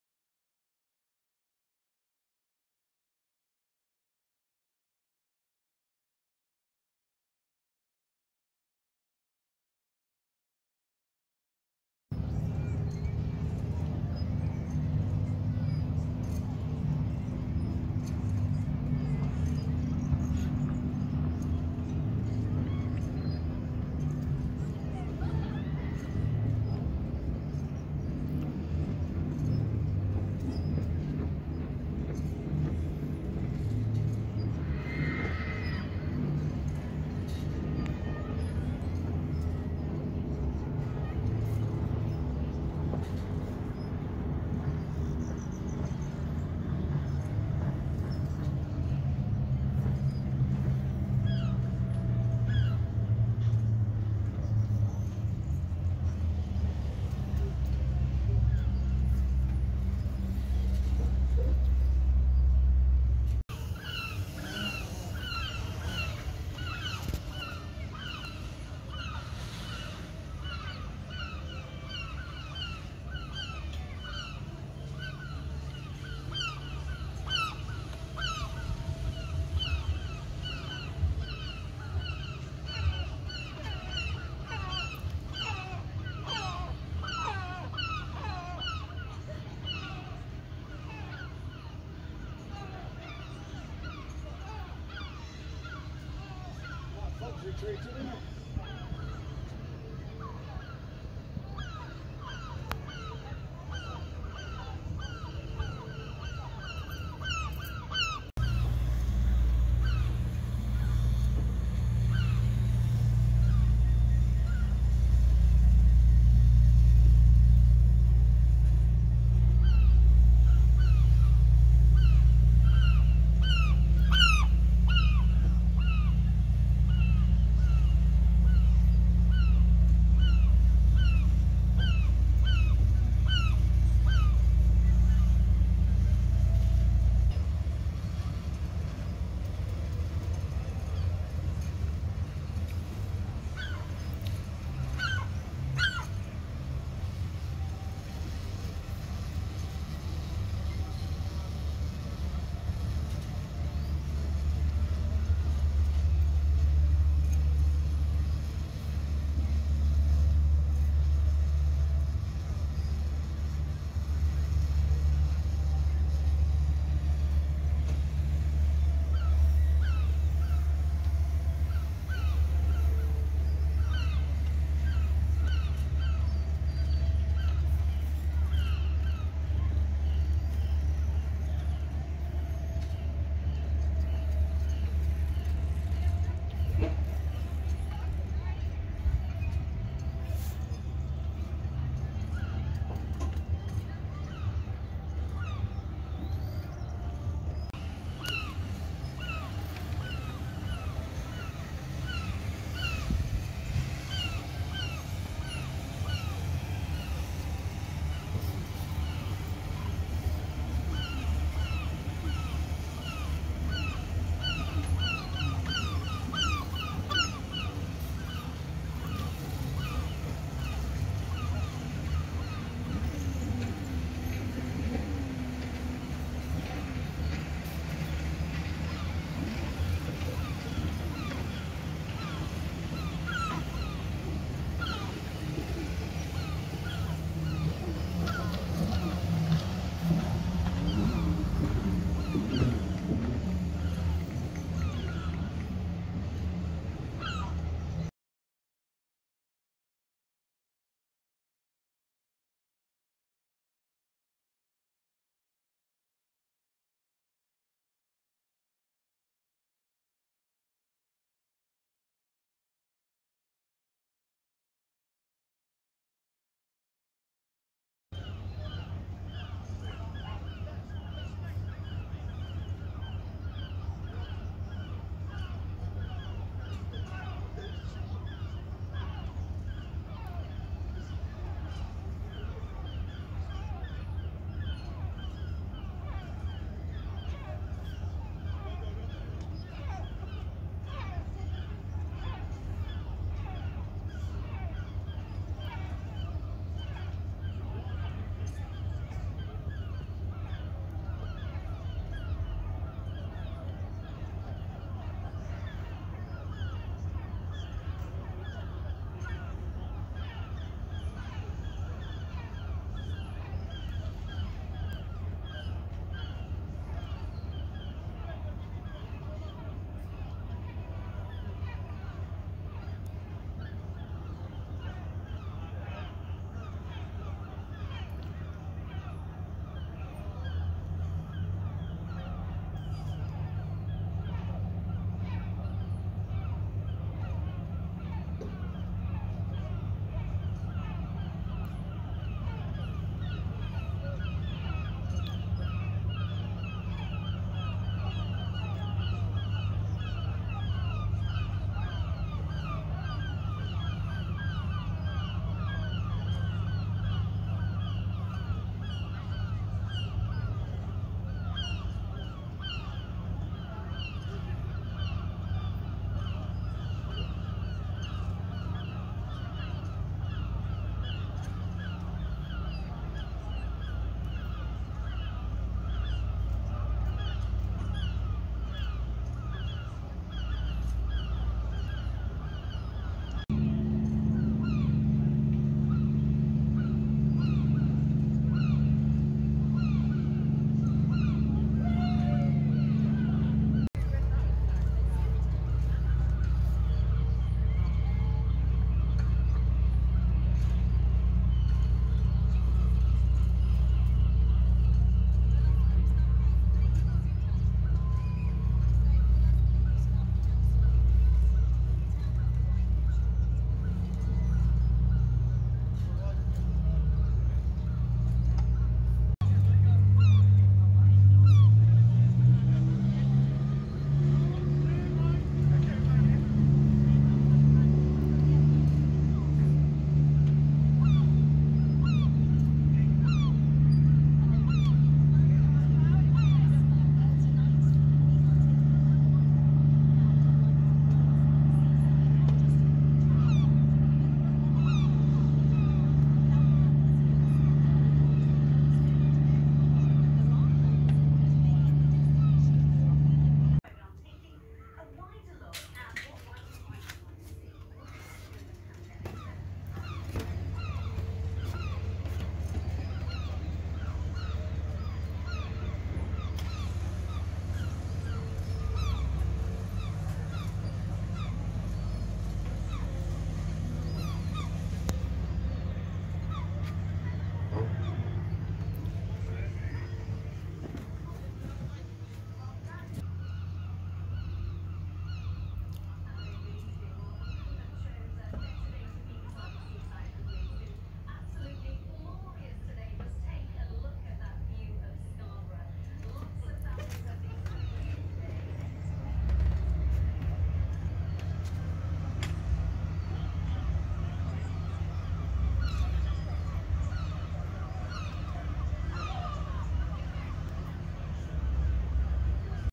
BBC in Scarborough
The BBC in Scarborough all week, with Radio 4, BBC News and Look North Yorkshire. Sorry Michelle Hussain not Nina.